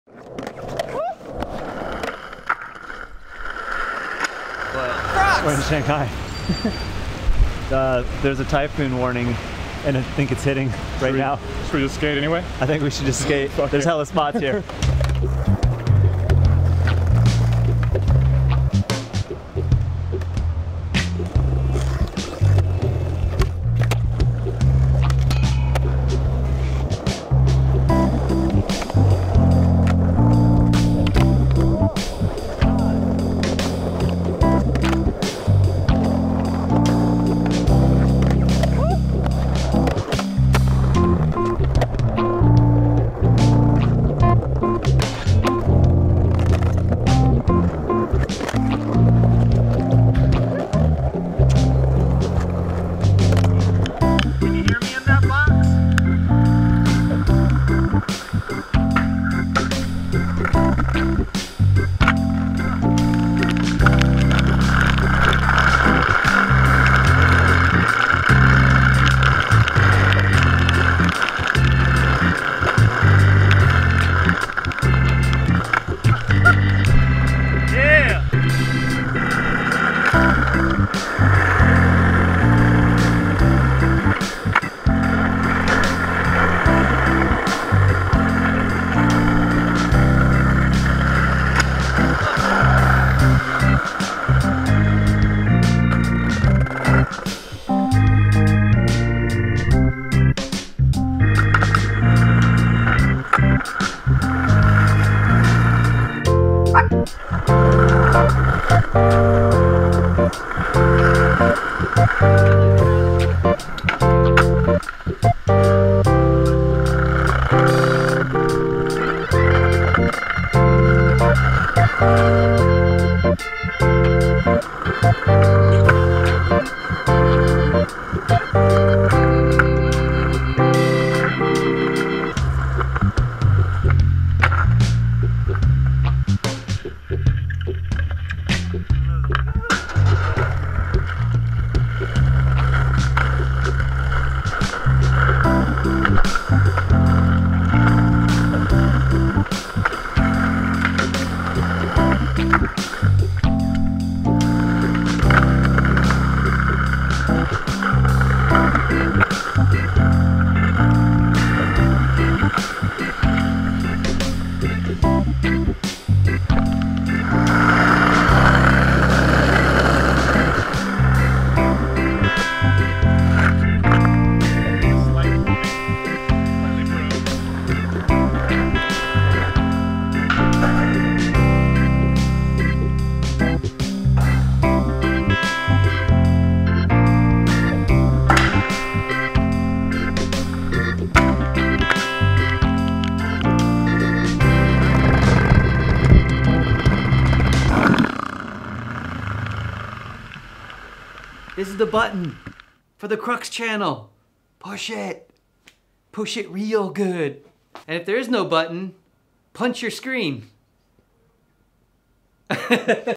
Skating in a Monsoon
Louie Barletta, Cairo Foster, Jimmy Carlin and Ron Whaley decide it would be a good idea to skate the soaked streets of Shanghai during a Monsoon.

Filmed by a well moistened Joe Perrin